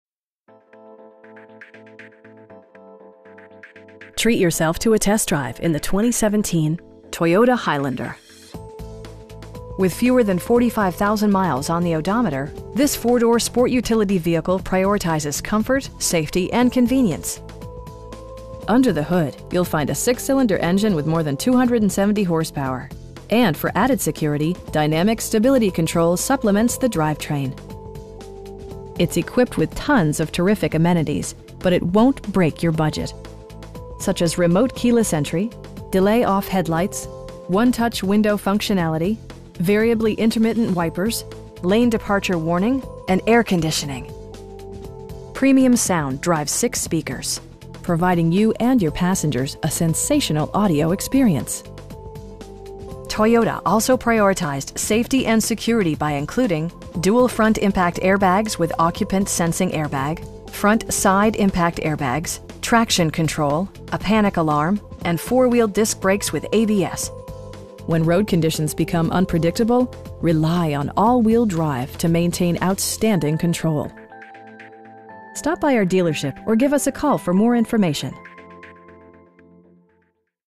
2017 Toyota Highlander in Riverdale, UT 84405
777 West Riverdale Rd.

Treat yourself to a test drive in the 2017 Toyota Highlander. With fewer than 45,000 miles on the odometer, this 4 door sport utility vehicle prioritizes comfort, safety and convenience. Under the hood you'll find a 6 cylinder engine with more than 270 horsepower, and for added security, dynamic Stability Control supplements the drivetrain. It's equipped with tons of terrific amenities, but it won't break your budget. Such as remote keyless entry, delay-off headlights, 1-touch window functionality, variably intermittent wipers, tilt and telescoping steering wheel, lane departure warning, cruise control, and air conditioning. Premium sound drives 6 speakers, providing you and your passengers a sensational audio experience. Toyota also prioritized safety and security by including: dual front impact airbags with occupant sensing airbag, front side impact airbags, traction control, brake assist, a panic alarm, and 4 wheel disc brakes with ABS. When road conditions become unpredictable, rely on all wheel drive to maintain outstanding control. You will have a pleasant shopping experience that is fun, informative, and never high pressured. Stop by our dealership or give us a call for more information.